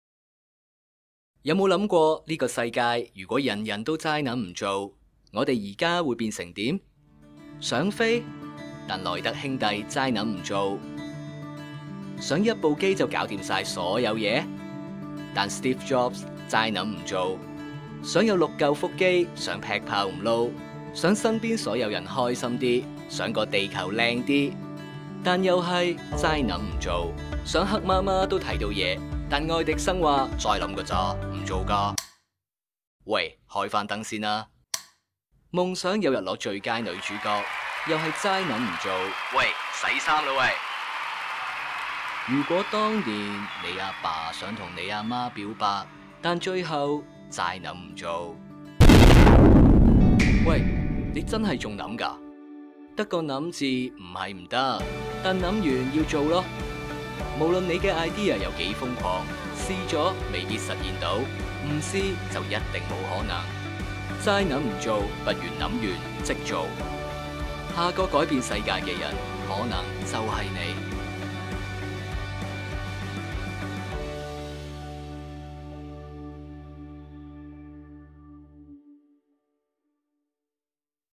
人人都齋諗唔做，個世界會變成點？ | Impact Innovation Lab 創效實現室
人人都齋諗唔做，個世界會變成點🌎？】

想飛？但萊特兄弟齋諗唔做。要請幾多日假先可以歐遊？

想一部機就搞掂所有嘢？但Steve Jobs齋諗唔做。呢刻你會用緊咩電話？

無論諗法有幾瘋狂，又或者依家聽落幾微不足道，雖然未知做咗可以去到幾遠，但齋諗就咩都無可能發生。

Impact Innovation Lab邀請你同我哋一齊實現想法，行多一步，可能下個影響世界嘅人就係你。

【If everyone keeps thinking without action, what will the world be like?🌎】

Dream of taking to the sky? If the Wright Brothers kept this thought to themselves, how long would a trip to Europe be?

Dream of having an all-in-one phone for doing different tasks? If Steve Jobs gave up the idea of the iPhone, what kind of device would we use now?

No matter how crazy or small your ideas sound, you never know what you will miss if you don’t make it happen.

Together, Impact Innovation Lab invites you to turn your vision into reality. Become the next person to impact the world!